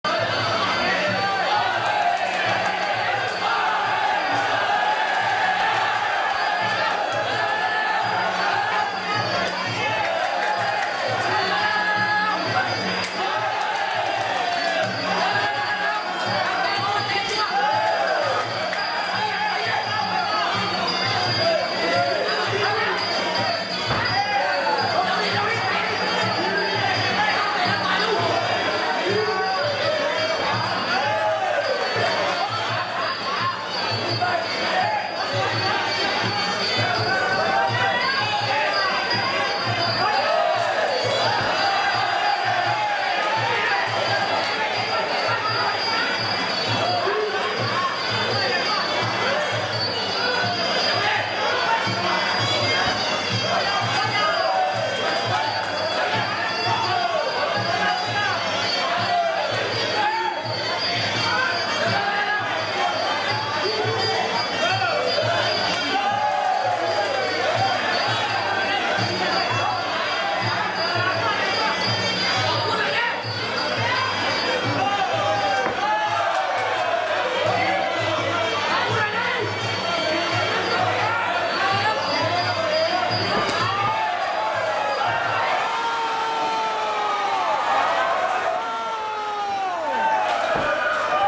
Italy vs. Thai KNOCKOUT Fight!!
This fight could go any way, who will knock out who, who will you put your money on??